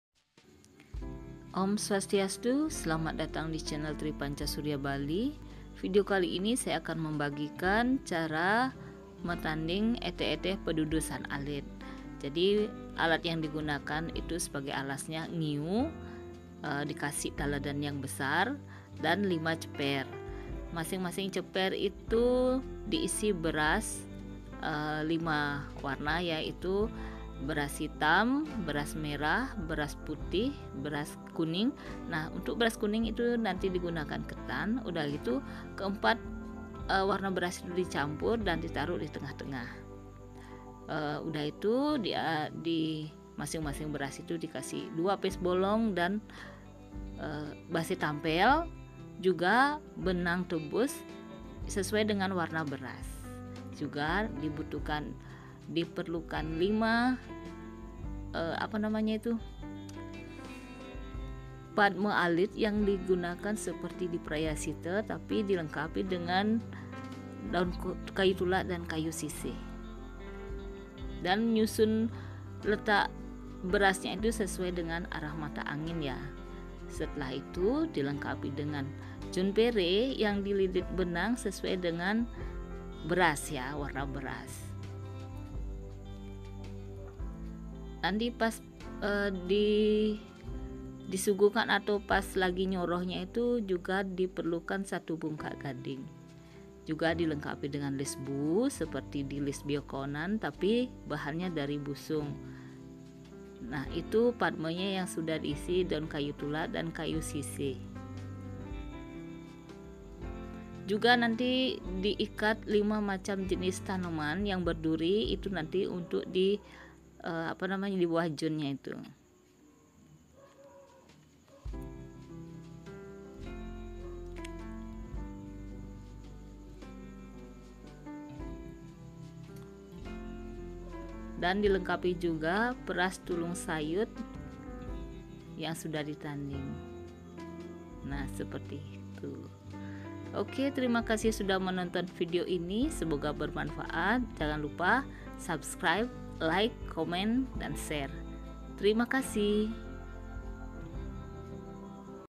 Metanding eteh-eteh pedudusan alit
Eteh-eteh pedudusan ini, sebgai sarana upacara yadnya, biasanya sebagai pelengkap Ida Sulinggih melaksanakan/ nganteb suatu upacara yadnya.
Video kali ini, secara singkat menyajikan metanding eteh-eteh pedudusan tersebut, bila ada kekeliruan dan kesalahan mohon dikoreksi, terimakasih🙏.

Ida sulinggih
beras merah
beras hitam
beras putih
benang tebus
padma alit
sarana upacara yadnya
don kayu tulak
don kayu sisih